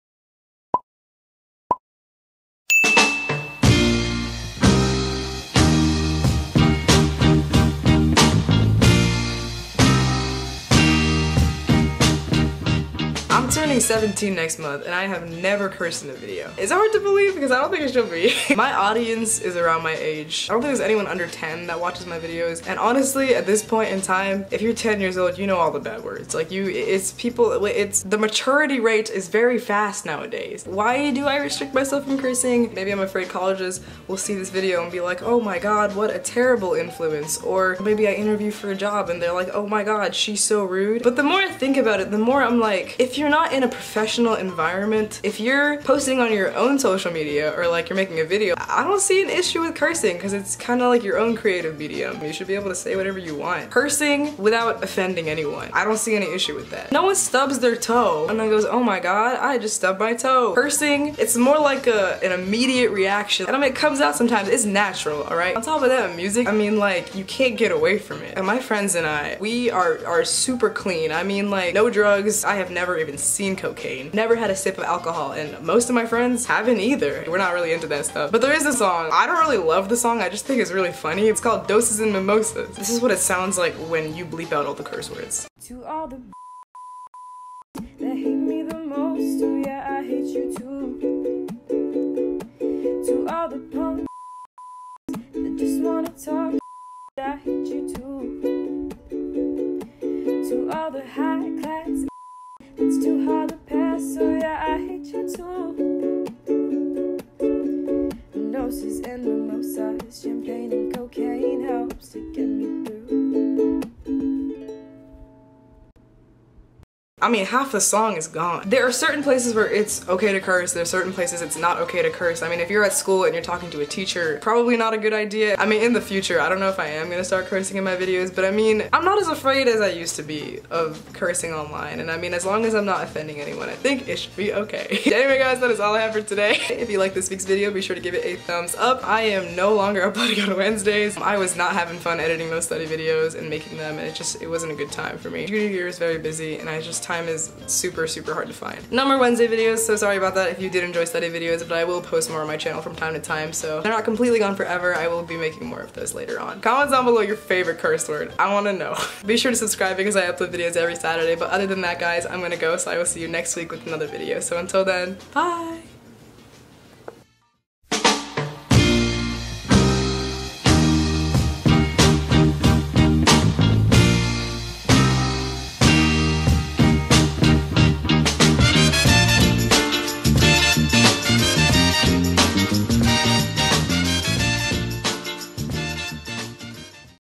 "CURSING IS INAPPROPRIATE"
Tried something a little different this week LOL

Cursing is expressive and without excessive use I don't think there's any issue with it.

I'm usually extremely cautious about cursing online, but honestly I'm starting to steer away from the idea that it's such a bad thing to curse.

When you're not in a professional environment, express yo self.

Comment your favorite cuss word down below LOL

Young-Holt Unlimited-